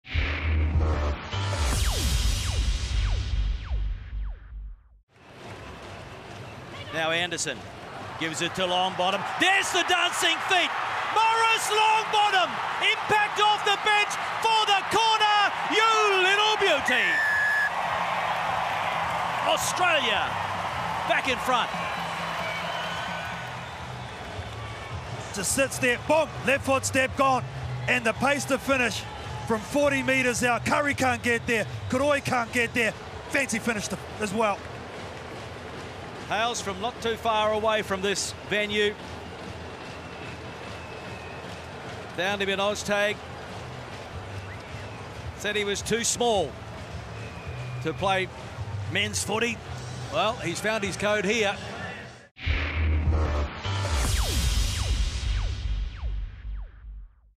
RE:LIVE: Maurice Longbottom
Australia's Maurice Longbottom announces his arrival onto the Cup quarter-final stage in style at the Sydney sevens.